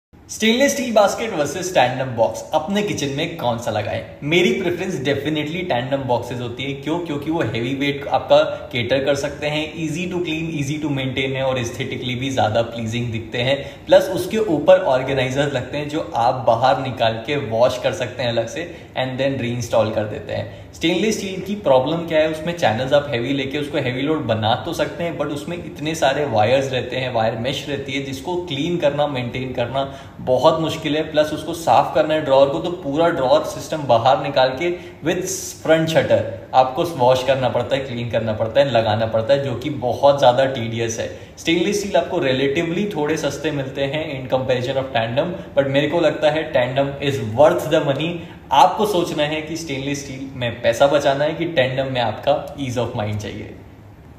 SS Basket v/s Tandem Drawers- Guide By HOUMEINDIA
You will see in this video SS Basket v/s Tandem Drawers- Guide By HOUMEINDIA

Embark on a comprehensive journey through kitchen organization with HOUMEINDIA in our latest YouTube video, "SS Basket vs. Tandem Drawers." This illuminating guide serves as your compass in navigating the choices between two popular storage solutions, ensuring you make an informed decision for an organized and efficient kitchen.

Join HOUMEINDIA as we unravel the distinctive features, benefits, and considerations of both SS Baskets and Tandem Drawers. Delve into the visual appeal, accessibility, and functionality each option offers, providing you with valuable insights to tailor your kitchen storage to your specific needs.

In this video, we address fundamental questions such as optimizing storage space, ease of access, and the impact on the overall kitchen aesthetic. HOUMEINDIA's expertise shines as we guide you through the decision-making process, empowering you to choose the storage solution that seamlessly integrates with your lifestyle and complements your kitchen design.

Whether you're renovating or planning a new kitchen, this guide ensures you embark on your project with clarity and confidence. Click play to unravel the choices between SS Baskets and Tandem Drawers with HOUMEINDIA. kitchenorganization SSBasketVsTandemDrawers HOUMEINDIA 🍽️✨🏡

If you're looking to effectively and beautifully design your dream house, we're here to help!